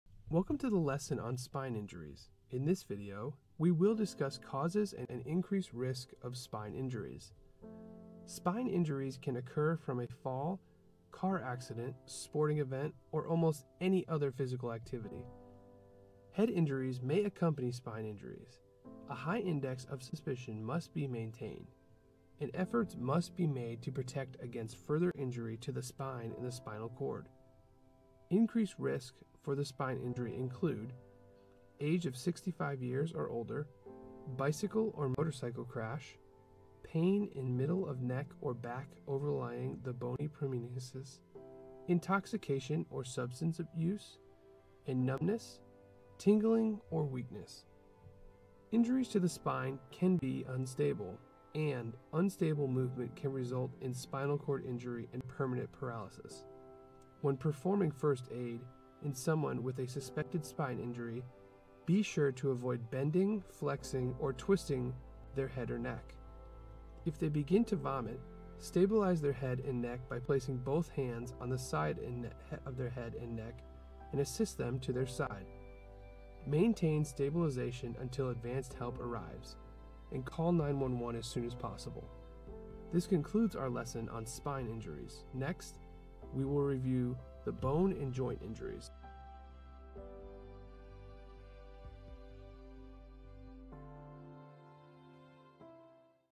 3g. Spine Injuries (CPR) (2018) OLD
The seventh lesson in chapter three is about Spine Injuries. Almost any type of physical activity can lead to a possible spine injury. Some people are at increased risk for these injuries, whether because of their age or other circumstances. Examples are given for these situations. How to stabilize a person with a spine injury and appropriate treatment is discussed.

"Spine injuries can occur from a fall, car accident, sporting event, or almost any other physical activity. Head injuries may accompany spine injuries. A high index of suspicion must be maintained, and efforts must be made to protect against further injury to the spine and the spinal cord.

Increased risks for spine injury include: age of 65 years or older, bicycle or motorcycle crash, pain in middle of neck or back overlying the bony prominences, intoxication or substance use, and numbness, tingling, or weakness.

Injuries to the spine can be unstable; and, unstable movement can result in spinal cord injury and permanent paralysis. When performing first aid in someone with a suspected spine injury, be sure to avoid bending, flexing, or twisting their head or neck. If they begin to vomit, stabilize their head and neck by placing both hands on the side of the head and neck, and assist them to their side. Maintain stabilization until advanced help arrives, and call 911 as soon as possible."